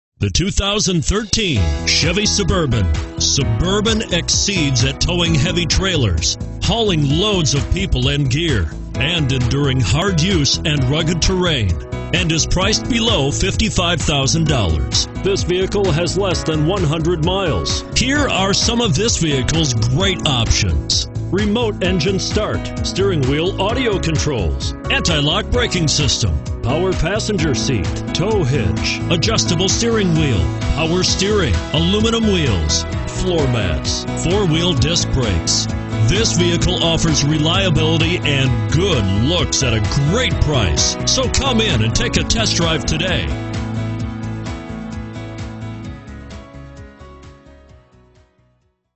2013 CHEVROLET SUBURBAN White Bear Lake, MN 83285
2013 CHEVROLET SUBURBAN White Bear Lake, MN
Stock# 83285

1801 E Co Road F

You'll love this 2013 Chevrolet Suburban. This is a car you'll want to take home. With 8 miles, it features Automatic transmission and an exterior color of CHAMPAGNE SILVER MET. Call us and be the first to open the car door today!